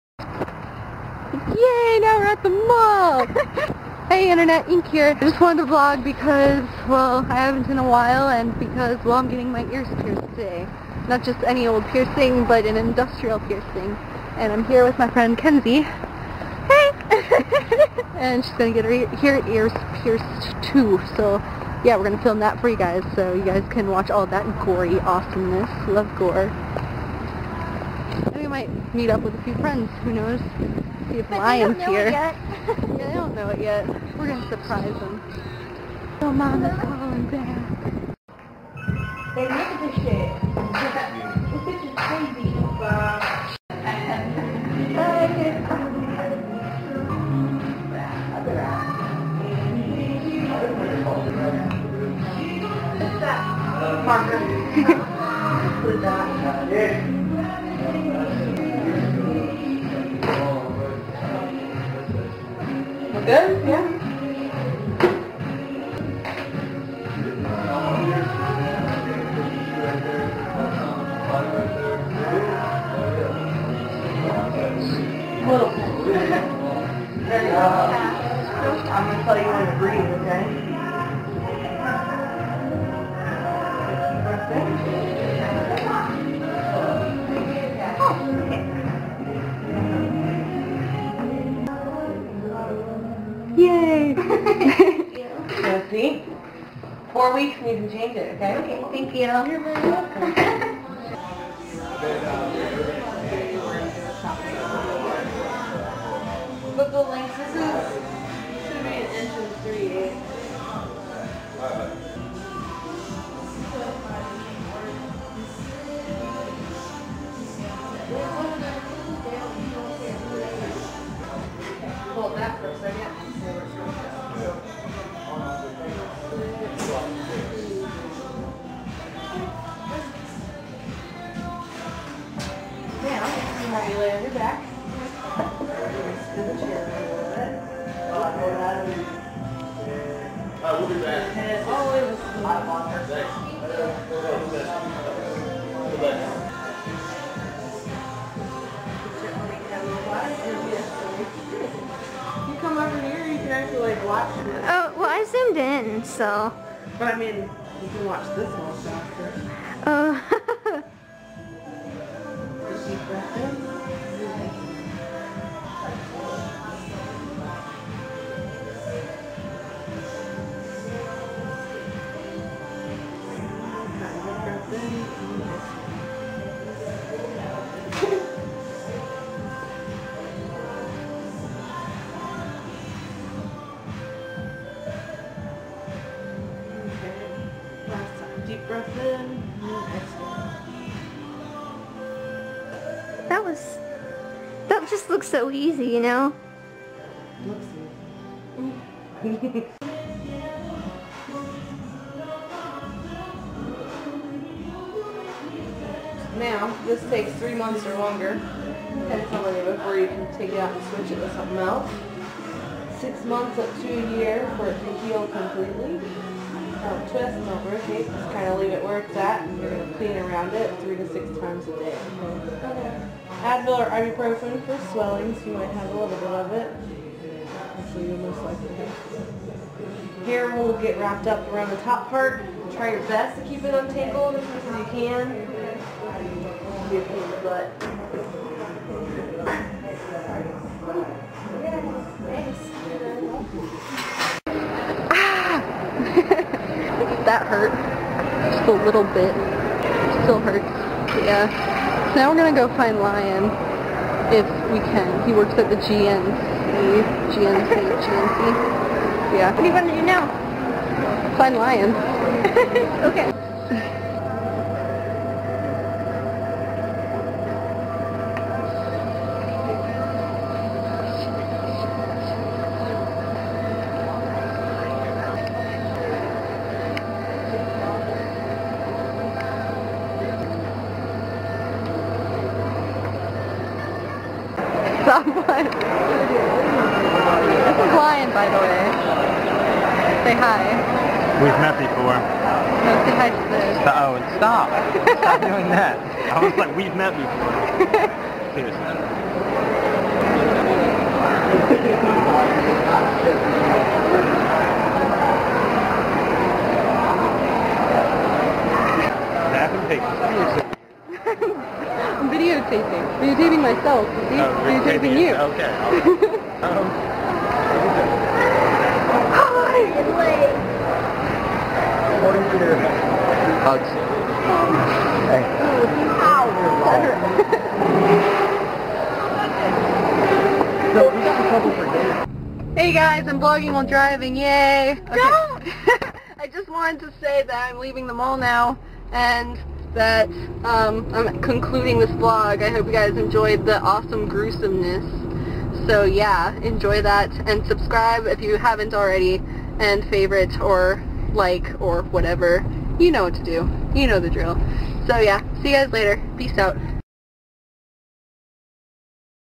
SWEET PIERCINGS!!! (3.2.12)
This really hurt!
But completely worth it.
I'll do a follow up video of how it's healing and give you guys some tips if you ever consider doing something like this yourself.